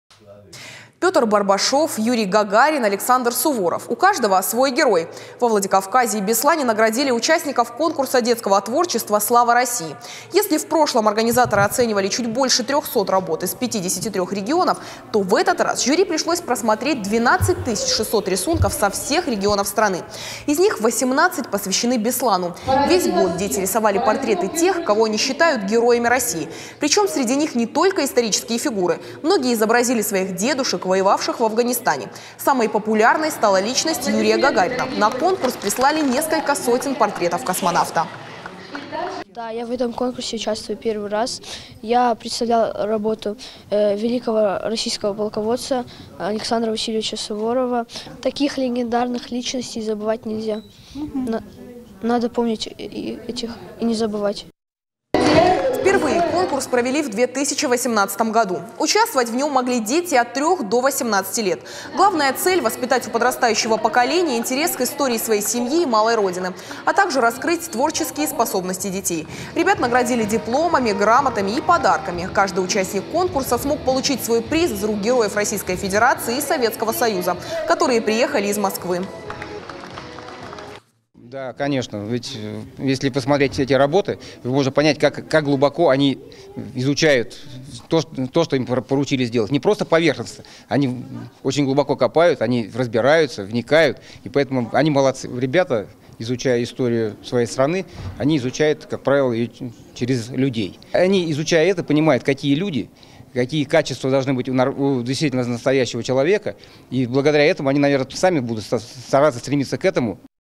Во Владикавказе наградили победителей конкурса детского творчества «Слава России»
Пётр Барбашов, Юрий Гагарин, Александр Суворов. У каждого – свой герой. Во Владикавказе и Беслане наградили участников конкурса детского творчества «Слава России».

Если в прошлом организаторы оценивали чуть больше 300 работ из 53 регионов, то в этот раз жюри пришлось просмотреть 12 тысяч 600 рисунков со всех регионов страны. Из них 18 посвящены Беслану. Весь год дети рисовали портреты тех, кого они считают героями России. Причём среди них не только исторические фигуры. Многие изобразили своих дедушек, воевавших в Афганистане. Самой популярной стала личность Юрия Гагарина. На конкурс прислали несколько сотен портретов космонавта.

Впервые конкурс провели в 2018 году. Участвовать в нём могли дети от 3 до 18 лет. Главная цель – воспитать у подрастающего поколения интерес к истории своей семьи и малой Родины, а также раскрыть творческие способности детей. Ребят наградили дипломами, грамотами и подарками. Каждый участник конкурса смог получить свой приз из рук героев Российской Федерации и Советского Союза, которые приехали из Москвы.
Мы рядом в:
ok.ru/irystontv